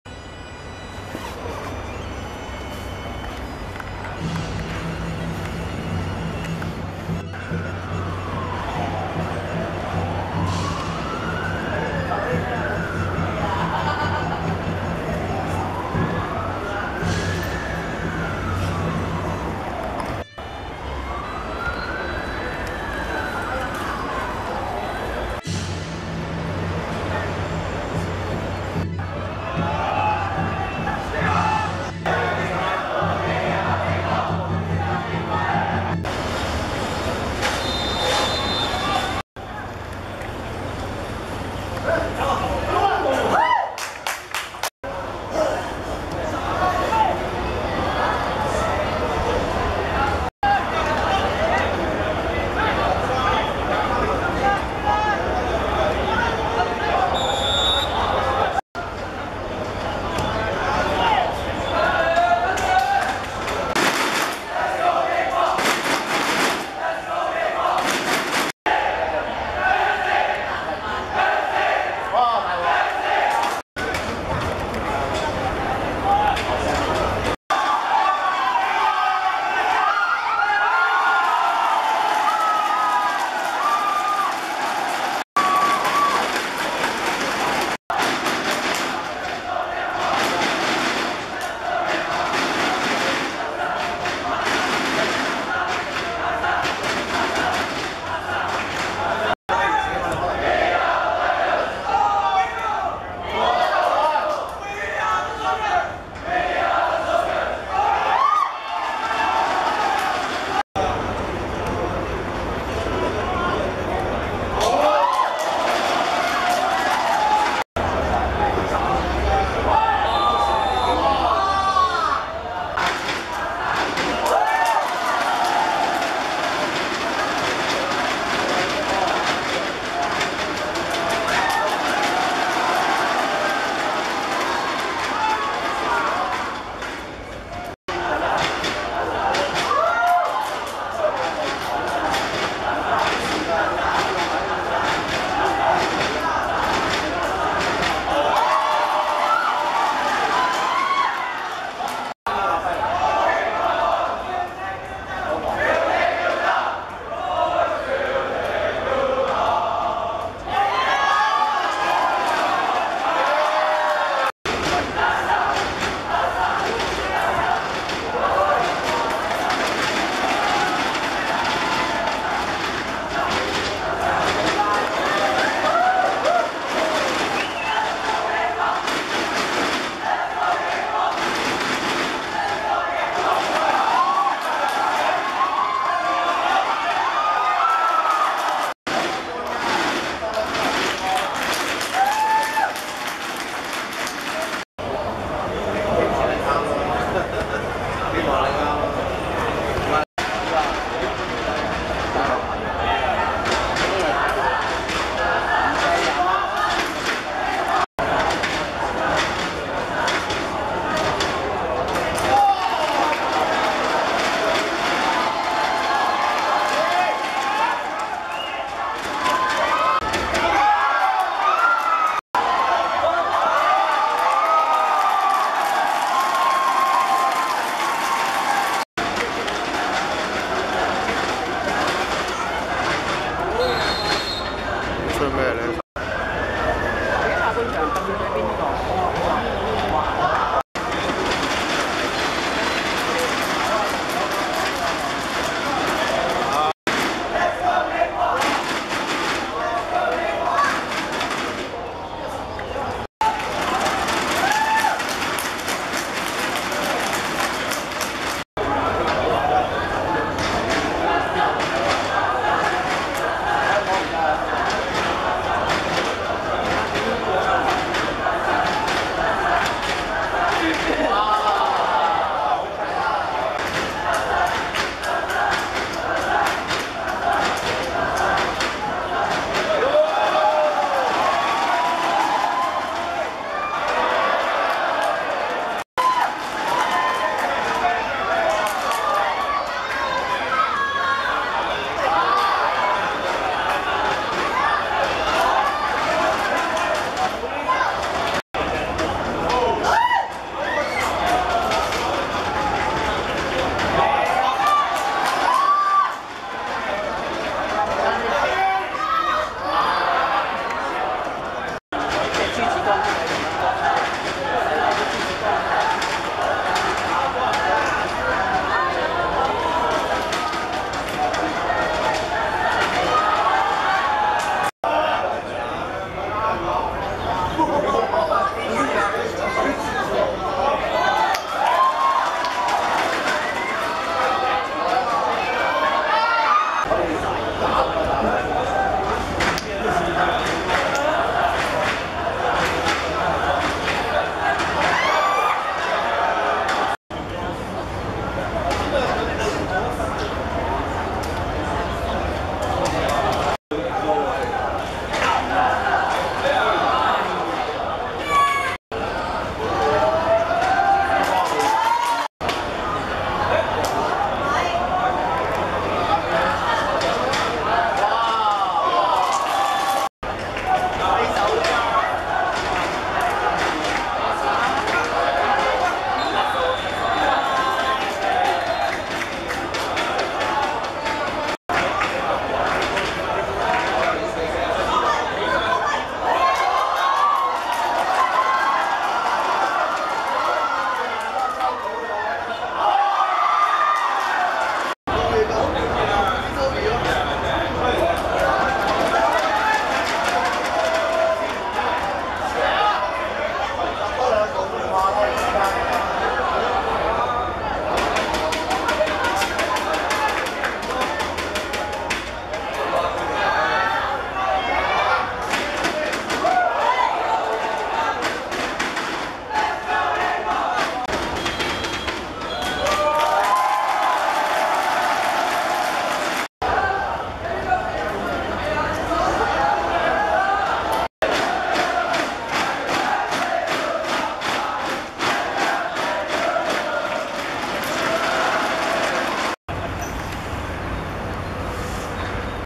英華vs喇沙(2014.3.12.學界足球精英賽準決賽)精華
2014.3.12.深水埗運動塲 15:00 英華(黃) 2:0 喇沙(白)